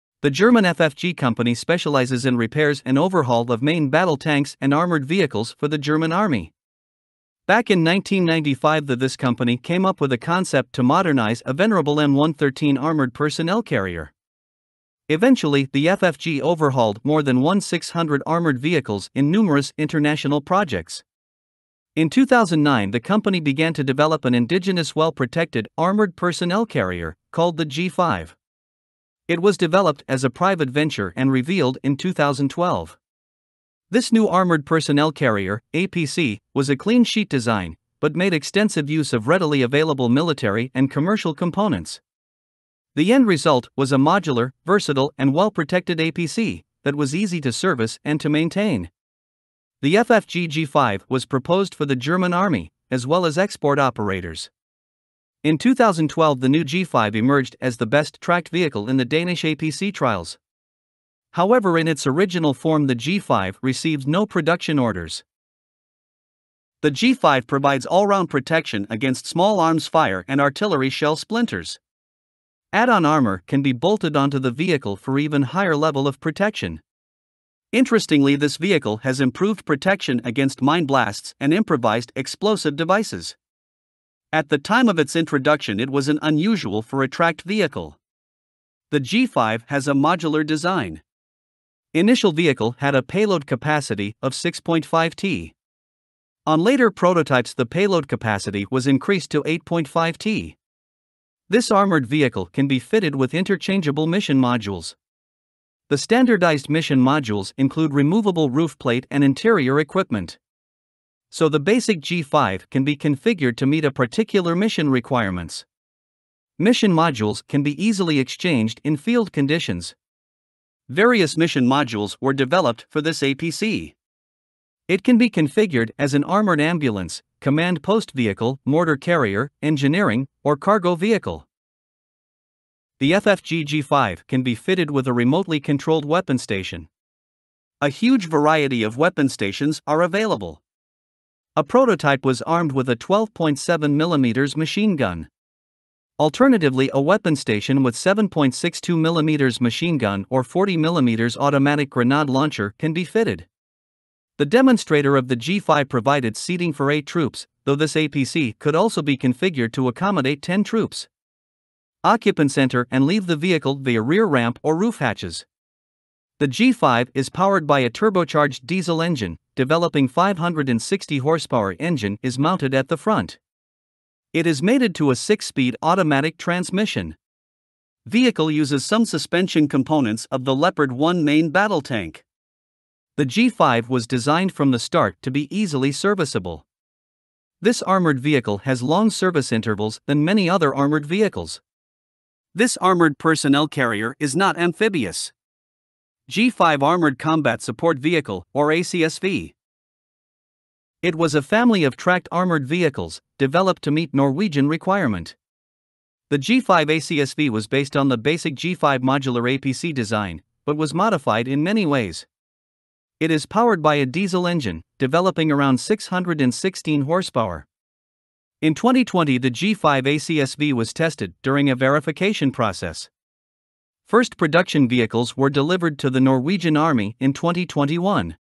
G5 Armored Personnel Carrier (Germany)
The German FFG company specializes in repairs and overhaul of main battle tanks and armored vehicles for the German Army. Back in 1995 the this company came up with a concept to modernize a venerable M113 armored personnel carrier. Eventually the FFG overhauled more than 1 600 armored vehicles in numerous international projects. In 2009 the company began to develop an indigenous well protected armored personnel carrier, called the G5. It was developed as a private venture and revealed in 2012. This new Armored Personnel Carrier (APC) was a clean sheet design, but made extensive use of readily available military and commercial components. The end result was a modular, versatile and well protected APC, that was easy to service and to maintain. The FFG G5 was proposed for the German Army, as well as export operators. In 2012 the new G5 emerged as the best tracked vehicle in the Danish APC trials. However in its original form the G5 received no production orders.

The G5 provides all-round protection against small arms fire and artillery shell splinters. Add-on armor can be bolted onto the vehicle for even higher level of protection. Interestingly this vehicle has improved protection against mine blasts and improvised explosive devices. At the time of its introduction it was an unusual for a tracked vehicle.

The G5 has a modular design. Initial vehicle had a payload capacity of 6.5 t. On later prototypes the payload capacity was increased to 8.5 t. This armored vehicle can be fitted with interchangeable mission modules. The standardized mission modules include removable roof plate and interior equipment. So the basic G5 can be configured to meet a particular mission requirements. Mission modules can be easily exchanged in field conditions. Various mission modules were developed for this APC. It can be configured as an armored ambulance, command post vehicle, mortar carrier, engineering, or cargo vehicle.

The FFG G5 can be fitted with a remotely controlled weapon station. A huge variety of weapon stations are available. A prototype was armed with a 12.7 mm machine gun. Alternatively a weapon station with 7.62 mm machine gun or 40 mm automatic grenade launcher can be fitted.

The demonstrator of the G5 provided seating for 8 troops, though this APC could also be configured to accommodate 10 troops. Occupants enter and leave the vehicle via rear ramp or roof hatches.

The G5 is powered by a turbocharged diesel engine, developing 560 hp. Engine is mounted at the front. It is mated to a 6-speed automatic transmission. Vehicle uses some suspension components of the Leopard 1 main battle tank. The G5 was designed from the start to be easily serviceable. This armored vehicle has long service intervals than many other armored vehicles. This armored personnel carrier is not amphibious.

G5 Armored Combat Support Vehicle (or ACSV). It was a family of tracked armored vehicles, developed to meet Norwegian requirement. The G5 ACSV was based on the basic G5 modular APC design, but was modified in many ways. It is powered by a diesel engine, developing around 616 hp. In 2020 the G5 ACSV was tested during a verification process. First production vehicles were delivered to the Norwegian Army in 2021.